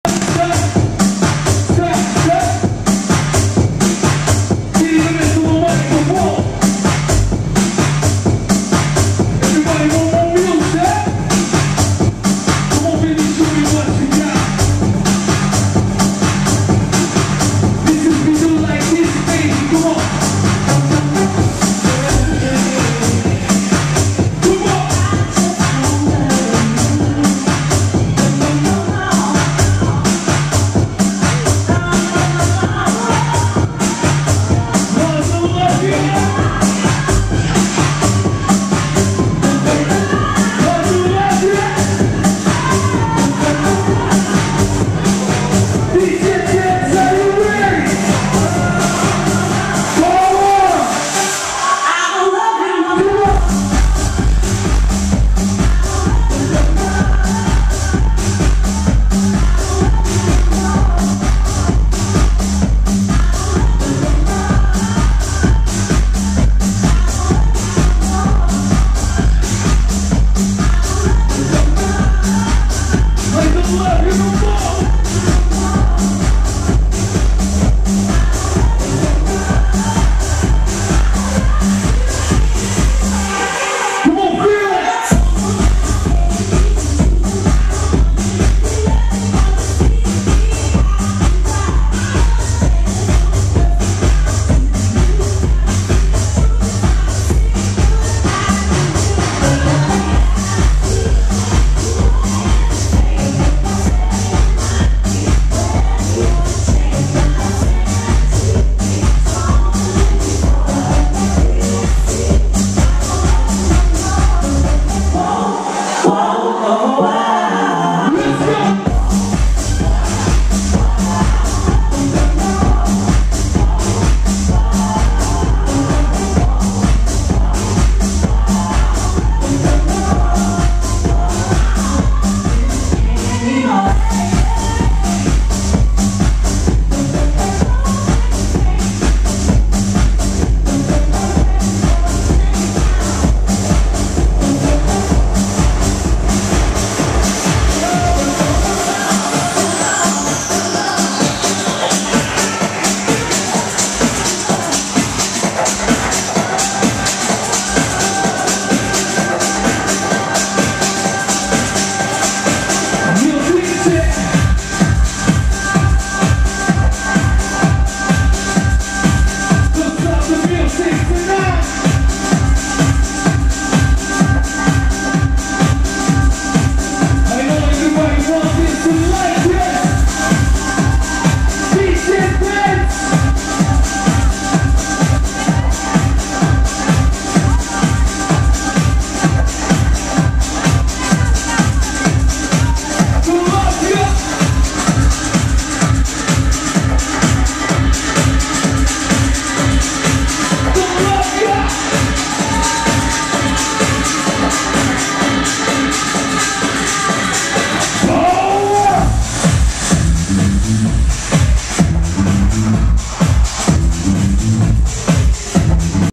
Boa Club Pole Dancer - Bucharest Romania
Crazy pole dancer at the Boa club in bucharest romania climbing to the sky